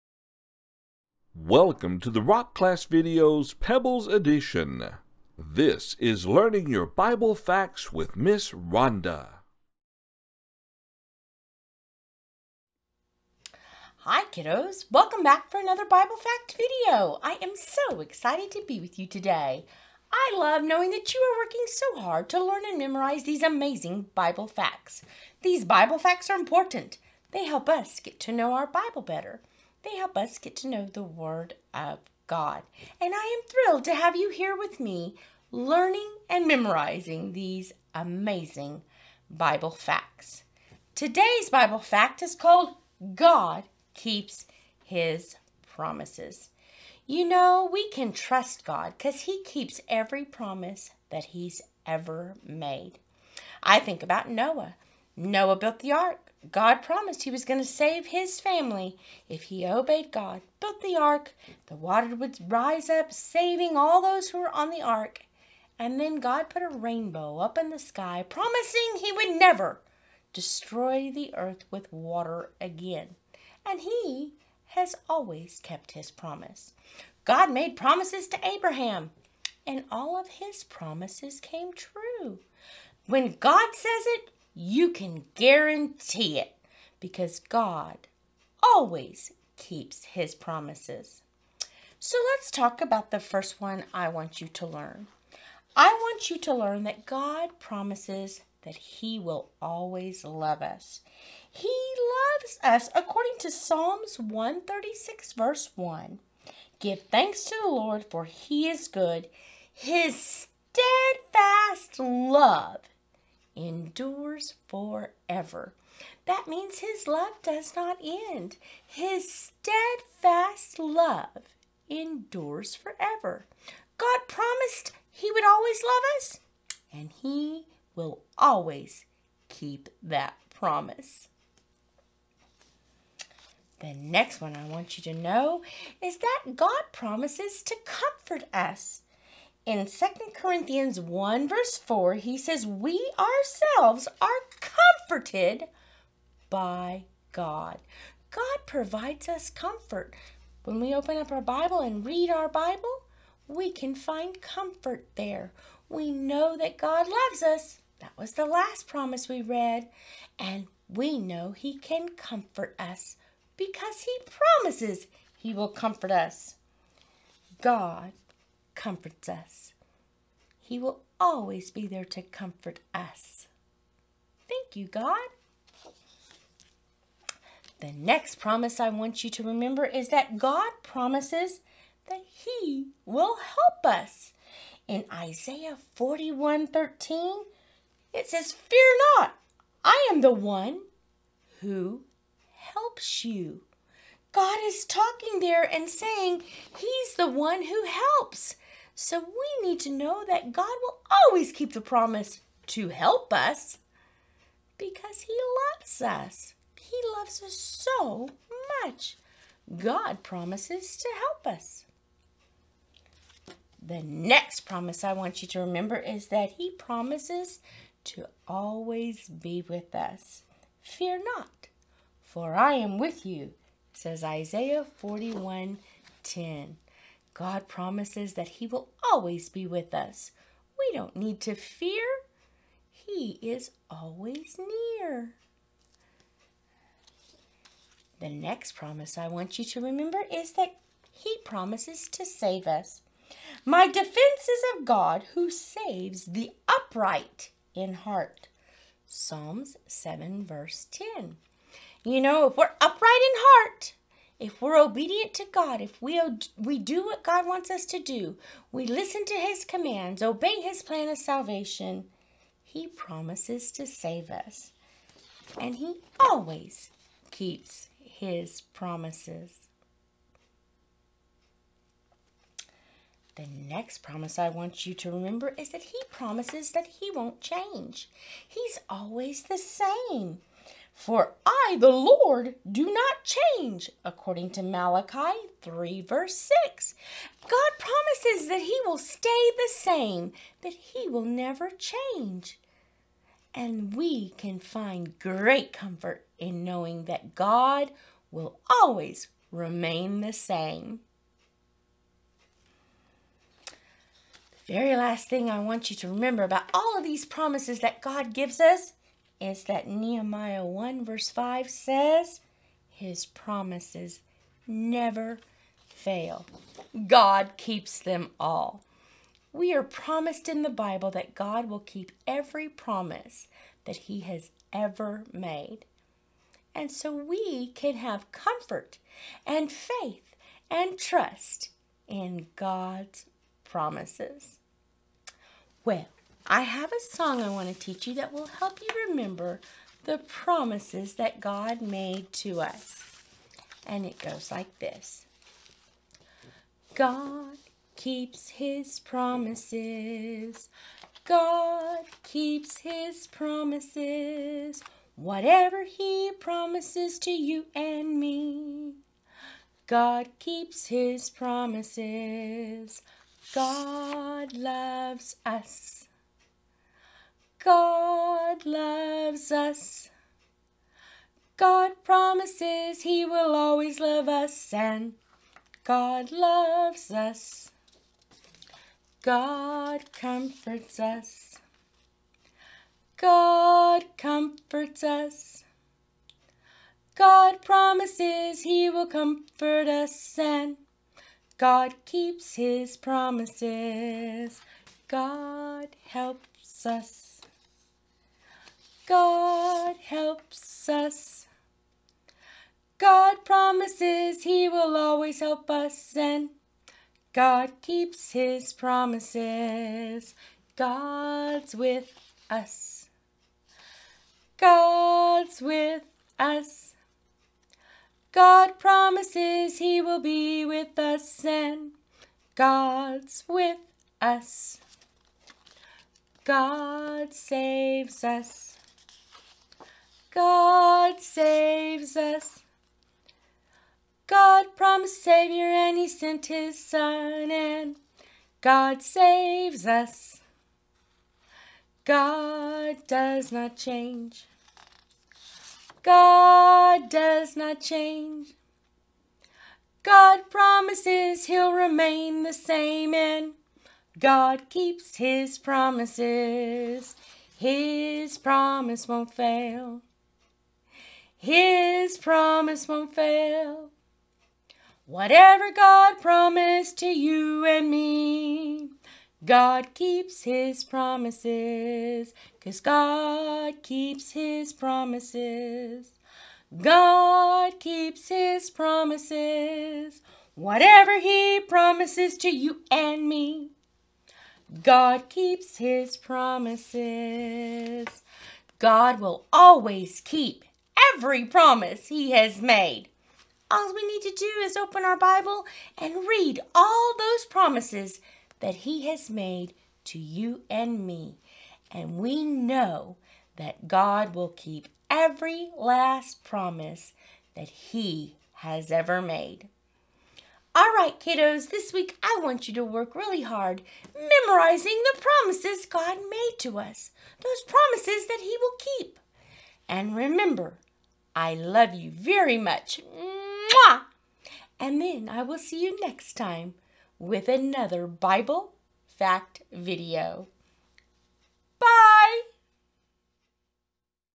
RCV - Pebbles - Learning Your Bible Facts - Ep 42 - God Keeps His Promises
In this video, Mrs. Ronda talks about how God keeps His promises.